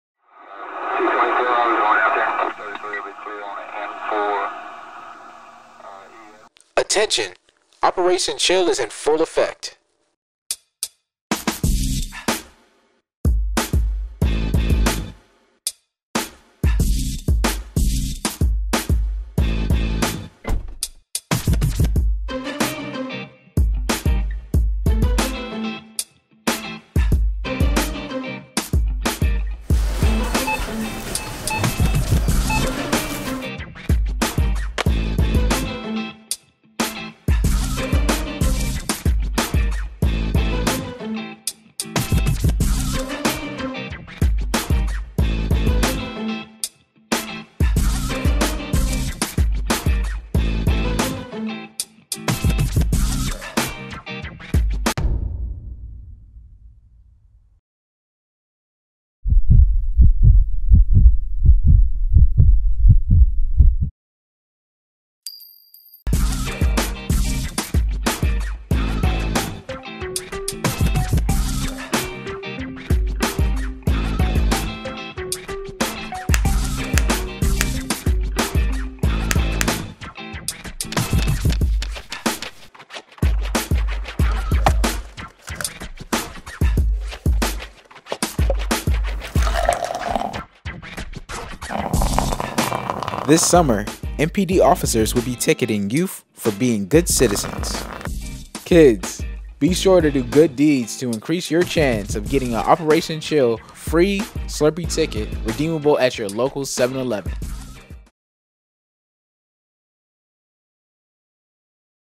***ATTENTION*** Operation Chill is in Full Effect (Get a Free Slurpee)
This summer, MPD officers will be "ticketing" or shall we say rewarding youth for being good citizens. Be sure to do good deeds to increase your chance of receiving an Operation Chill "free Slurpee" ticket, redeemable at a local 7-Eleven!

A special thank you to the children of the Marshall Heights summer camp, Sixth District Officers, the Sixth District Outreach unit, and 7-11 for their participation in making this video.